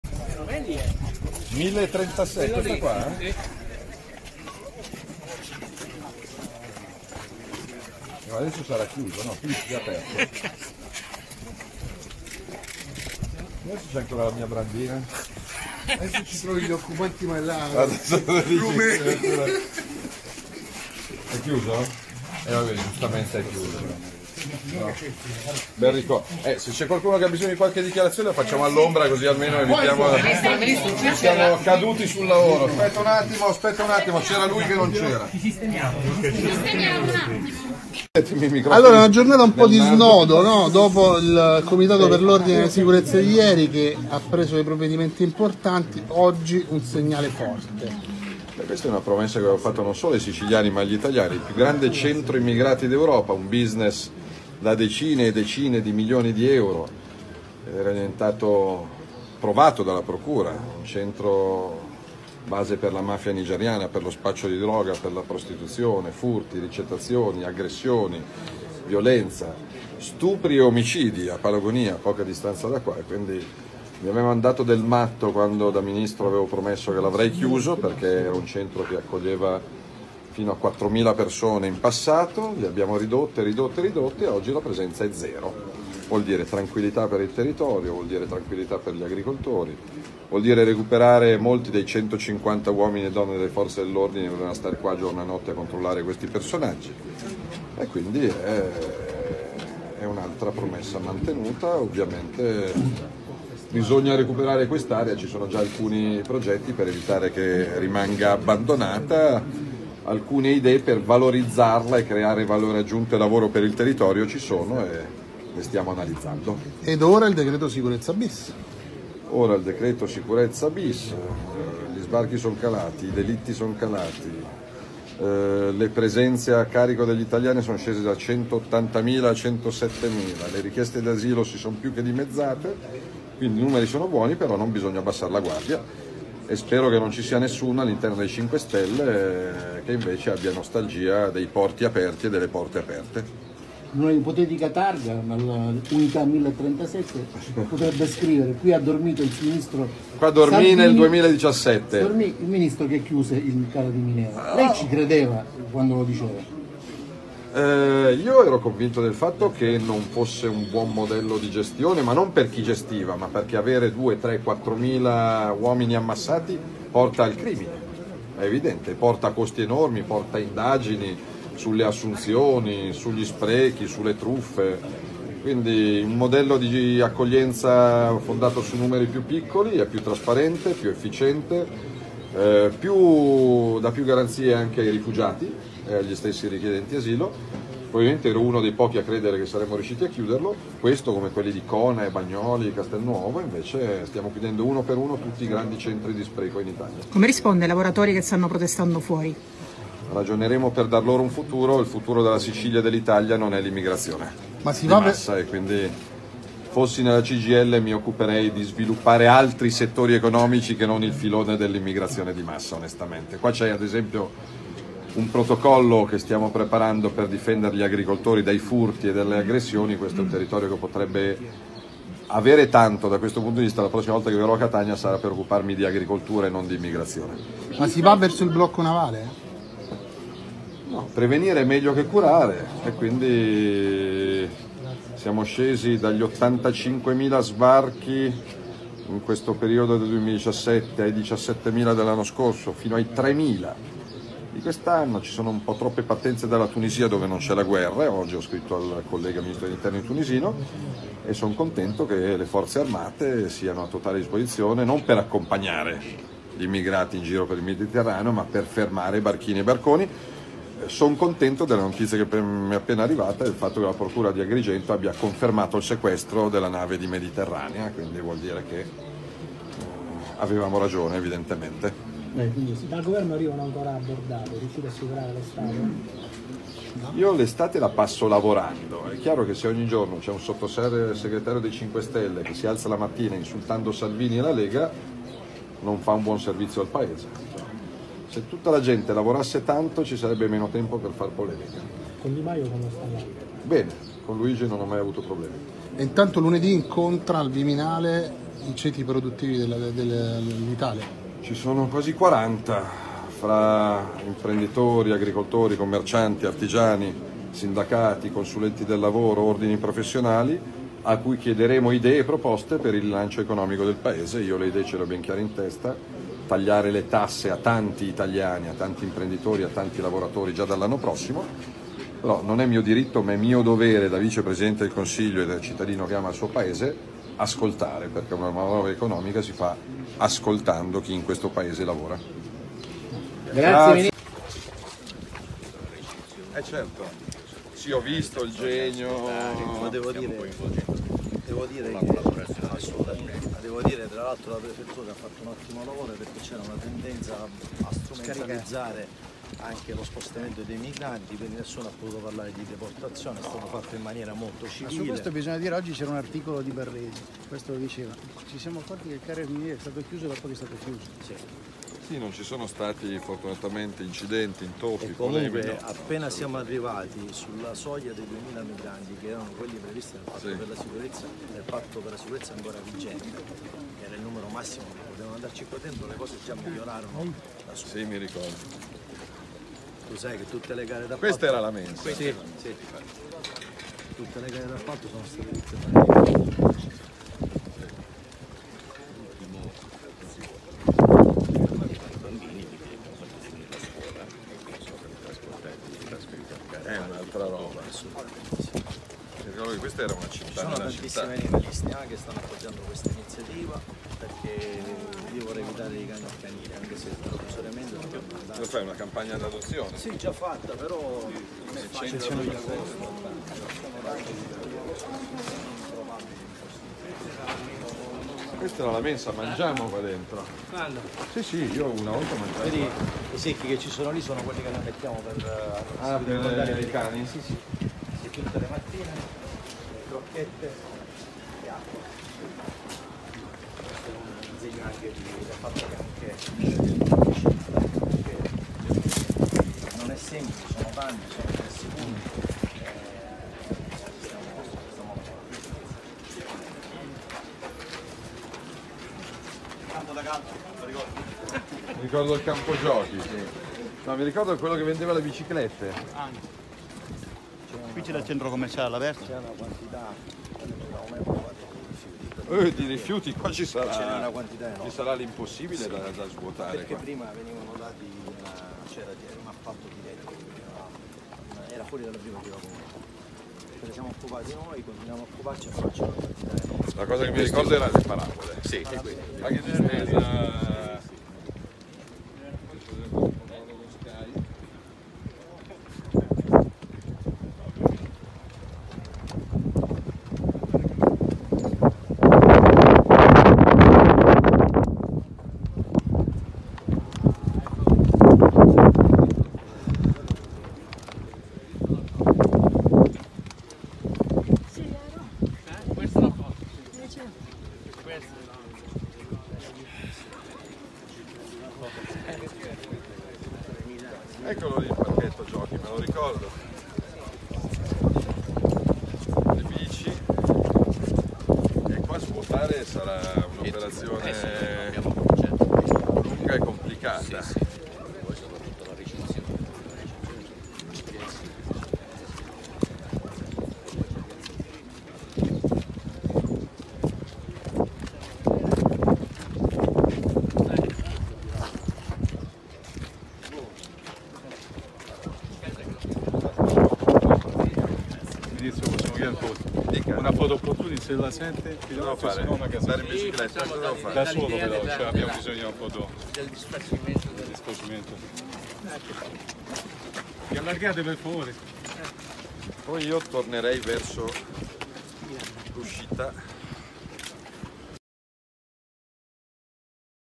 MATTEO SALVINI in diretta dall'ex CARA DI MINEO (09.07.2019)
Oggi è un bel giorno per la legalità!
Seguitemi, vi porto nell'ormai ex Cara di Mineo.